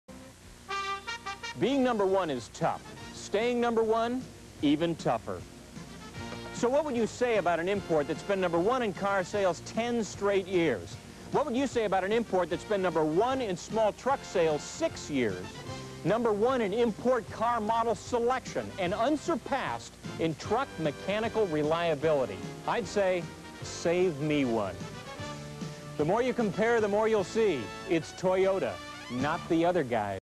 1985 Toyota Celica Supra Pickup TV Commercial
The commercial was originally aired on 2/1/1985. It was run during the Chicago Bulls Vs 76ers NBA Game on WGN Channel 9 in Chicago. WGN Channel 9 was an independent "SuperStation" broadcast nationally via satellite. The bulls lost 110 to 121